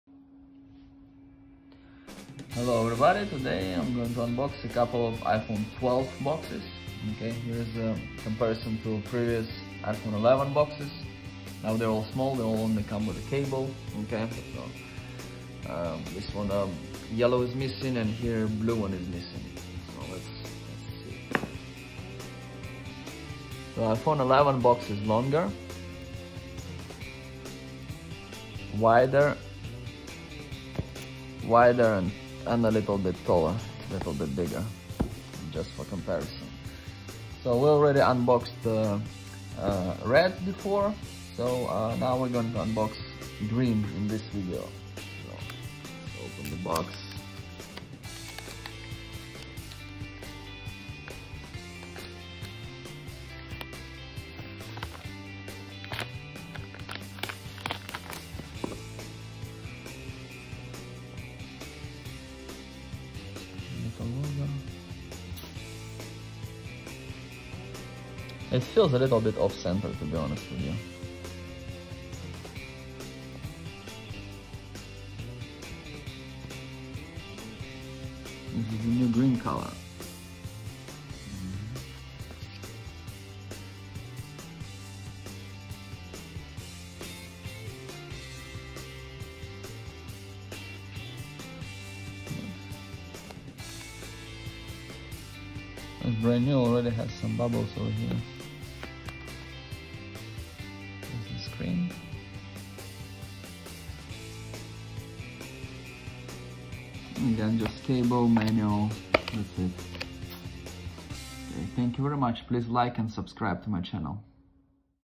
iPhone 12 Green - quick unboxing
Unboxing iPhone 12 Green
Comes in 64/128/256GB
Really Cute Color